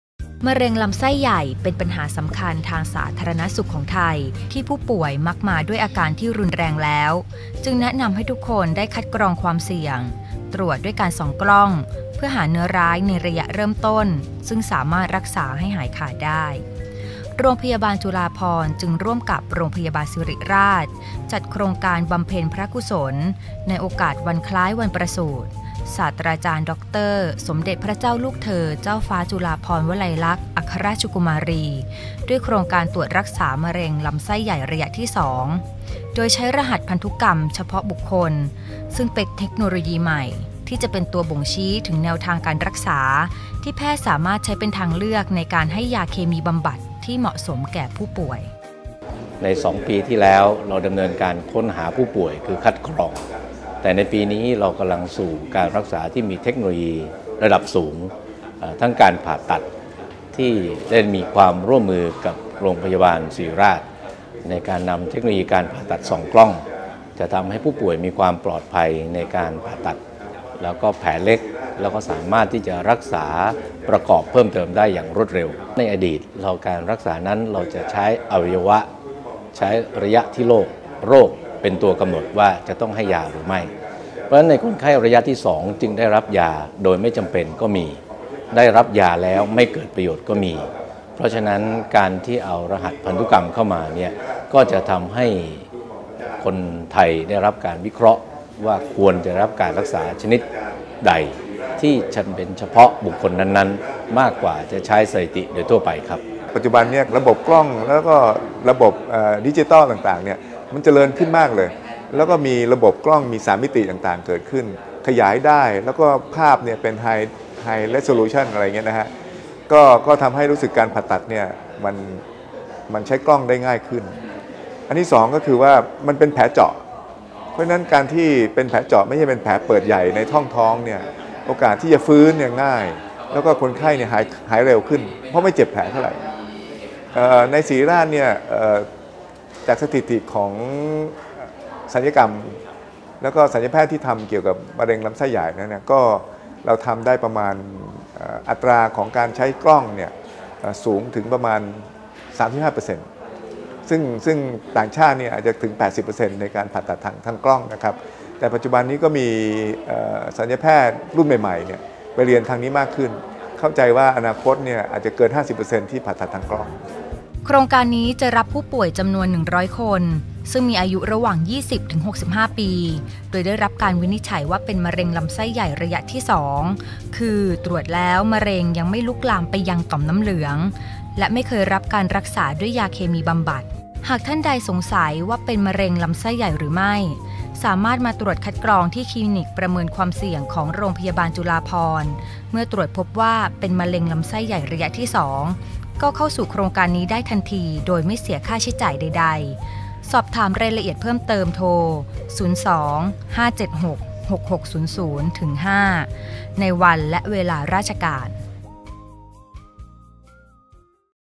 โครงการตรวจรักษามะเร็งลำไส้ใหญ่ระยะที่2 โดยใช้รหัสพันธุกรรมเฉพาะบุคคล
โดย โรงพยาบาลจุฬาภรณ์ ร่วมกับ โรงพยาบาลศิริราช
โทร. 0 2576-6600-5 ในวันและเวลาราชการ